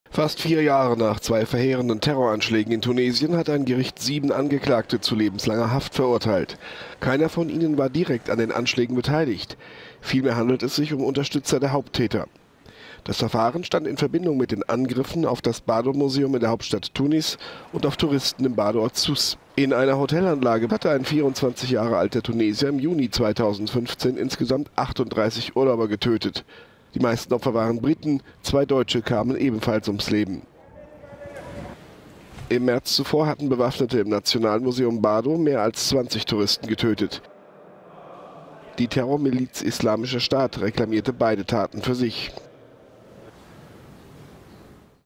Tunesien: Lebenslange Haft für Helfer der Attentäter von Sousse und Tunis
Vor gut vier Jahren trafen die Anschläge am Strand von Sousse und im Nationalmuseum Bardo in Tunesien das Land ins Mark. Jetzt hat ein Gericht sieben Männer lebenslang hintter Gitter geschiuckt. Doch sie sind nicht die Haupttäter.…